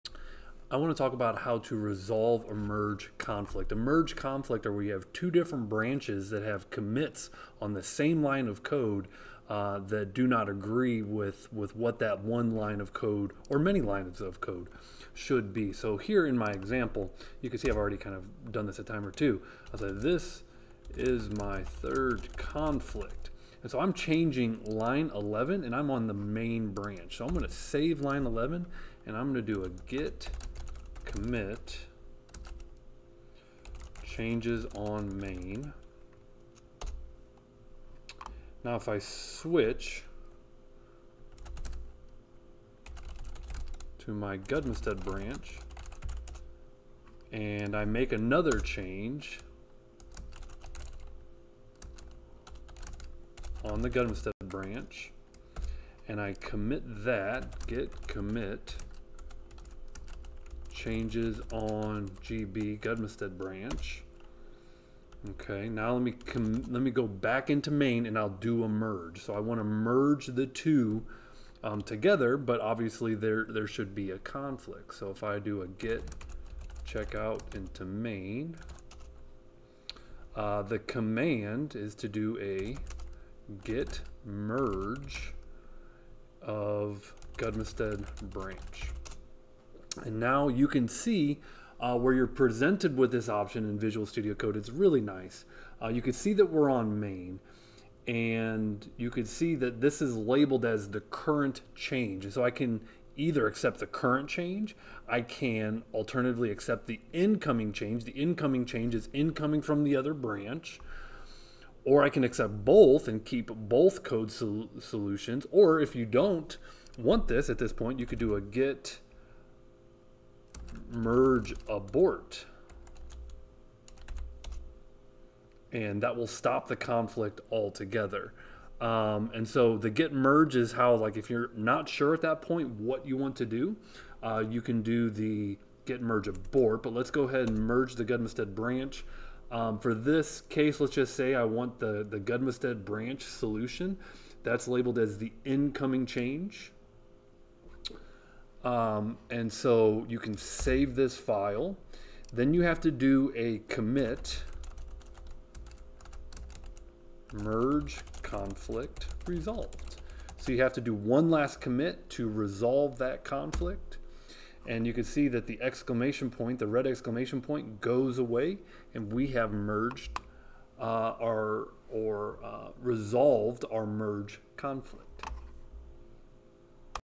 How to Resolve Merge Conflicts - Tutorial in VSCode
In this tutorial I show how to resolve merge conflicts with Git in VS Code